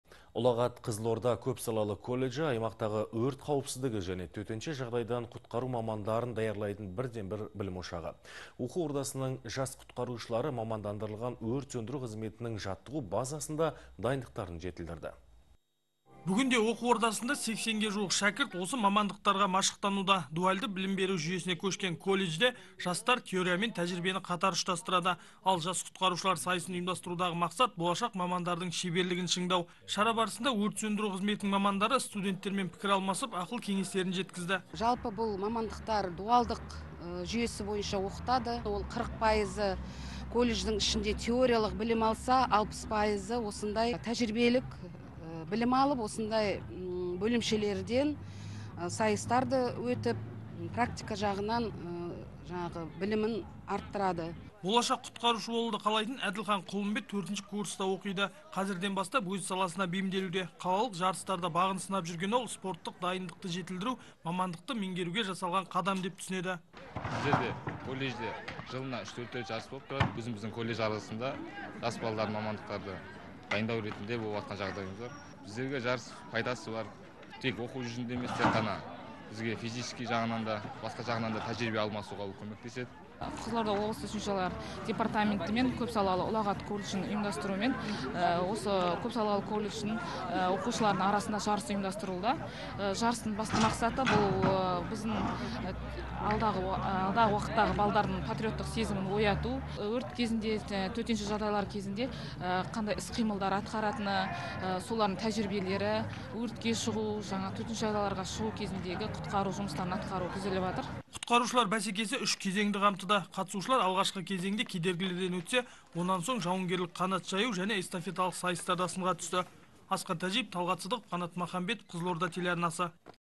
Жас құтқарушылар сайысы
«Ұлағат Қызылорда» көпсалалы колледжі аймақтағы «өрт қауіпсіздігі» және «төтенше жағдайдан құтқару» мамандарын даярлайтын бірден-бір білім ошағы. Оқу ордасының жас құтқарушылары мамандандырылған өрт сөндіру қызметінің жаттығу базасында дайындықтарын жетілдірді.